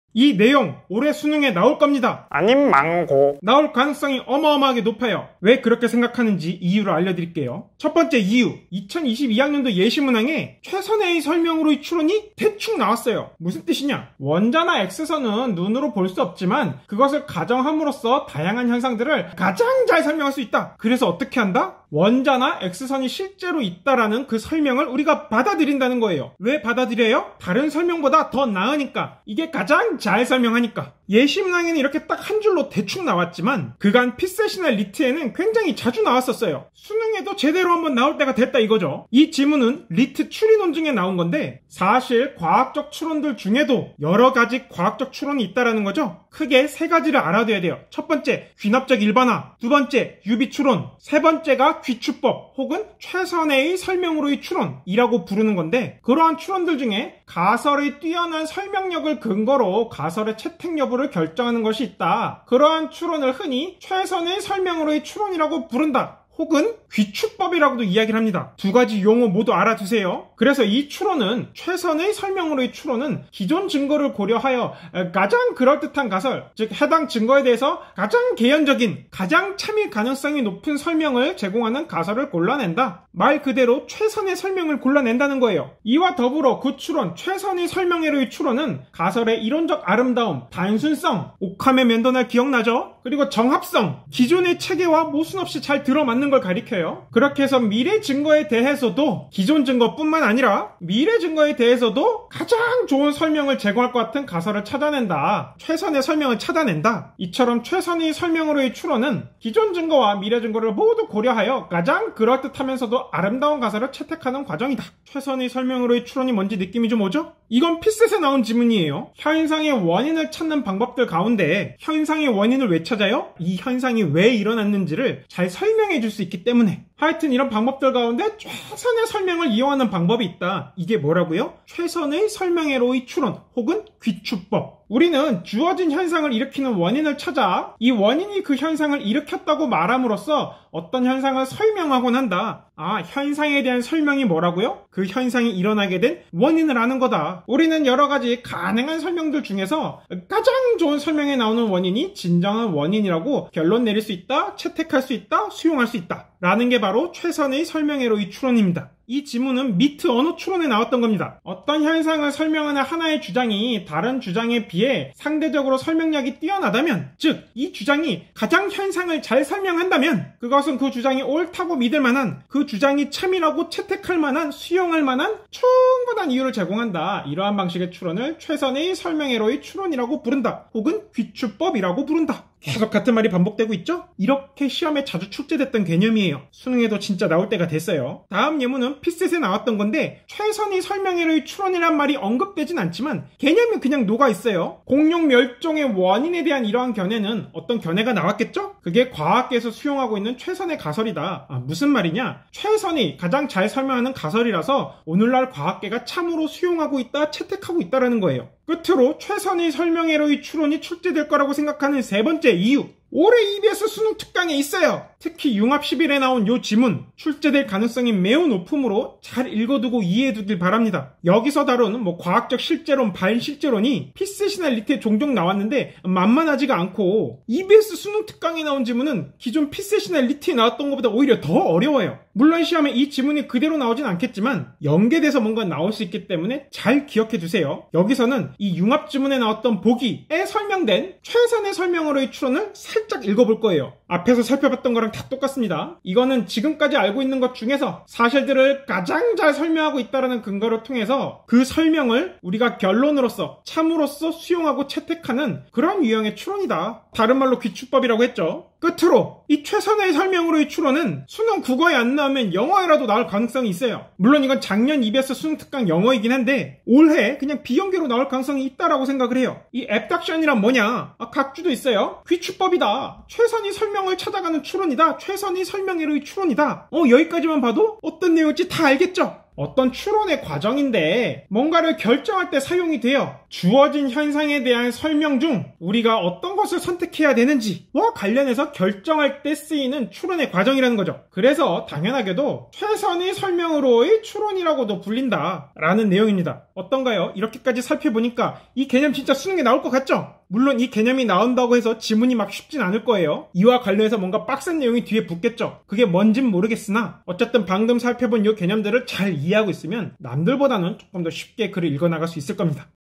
[오르비 클래스] [SWK6] 최선의 설명으로의 추론
수능 시험장에서 익숙한 느낌으로 풀어가고 싶다면?

2021학년도 수능과 가장 가까운,
2020년 시행 평가원 기출문제(독서) 분석 통해
수능 시험장에서 익숙한 논리구조 체험합시다.